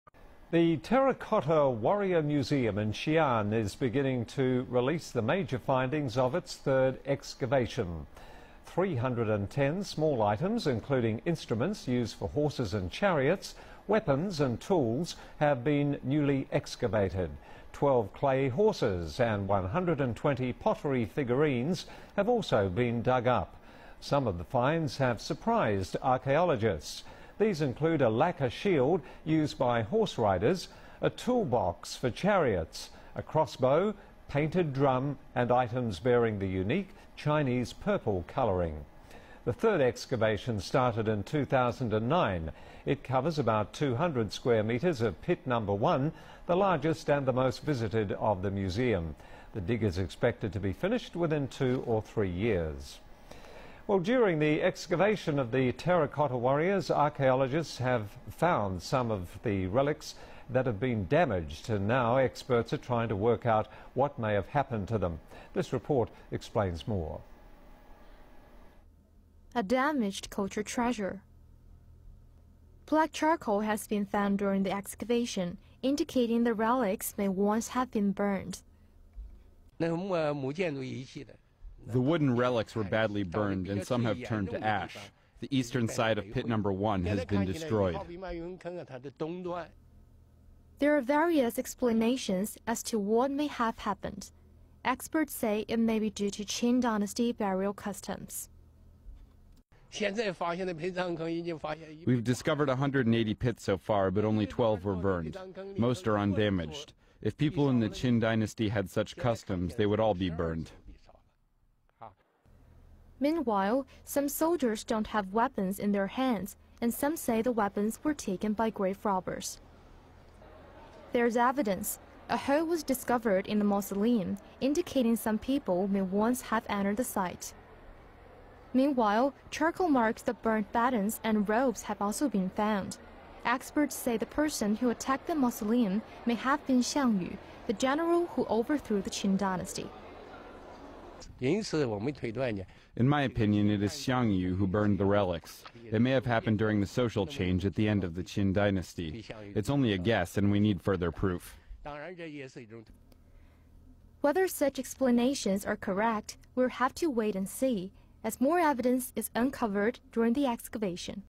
New secrets of the Terracotta Army
Xi'an's Terracotta Warrior Museum is beginning to release the major findings of its third excavation.